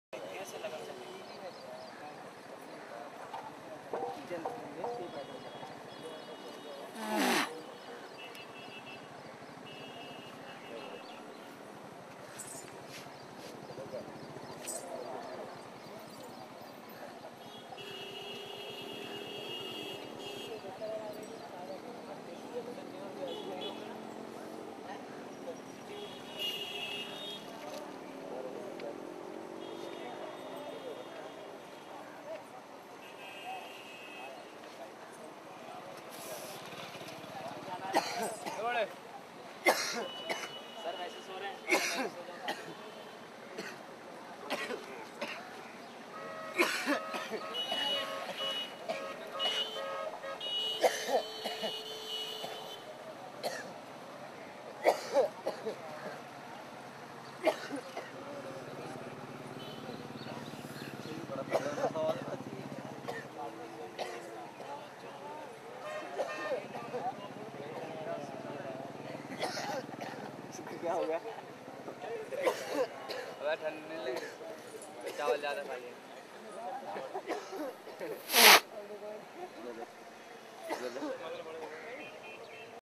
BHAGWAN SHIV KE BHKT SULFA -GANJA-CHRAS PEETEY
In Northern India according to hindu religion month of rain ie SAVAN ka mheena is dedicated to lord shiva also known as bholey baba .in this month long walk is done by deevoties from Haridwar to there home towns many hundred miles away they bring holy ganga water to spread over "shivling" in this long journey many deevoties inhale narcotics as lord shivas prshad.